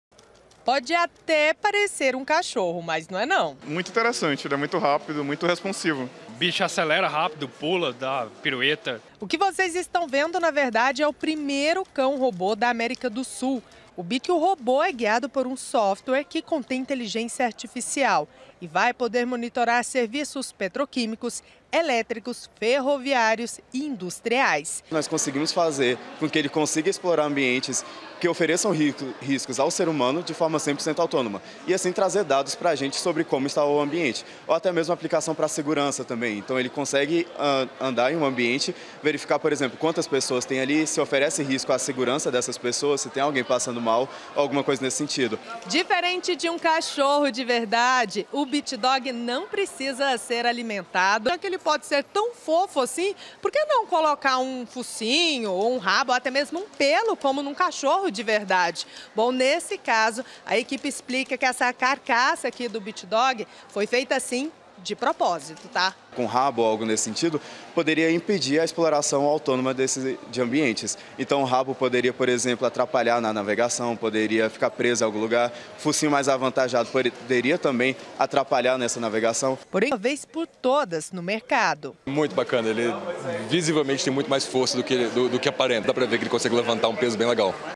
TECNOLOGIA: ESTUDANTES DA UFG CRIAM CÃO DO FUTURO
JÁ PENSOU TER EM CASA UM ANIMAL DE ESTIMAÇÃO QUE NÃO DÁ TRABALHO E NÃO FAZ SUJEIRA? É O CACHORRO DO FUTURO. ELE JÁ EXISTE E FOI DESENVOLVIDO POR ESTUDANTES DA UFG.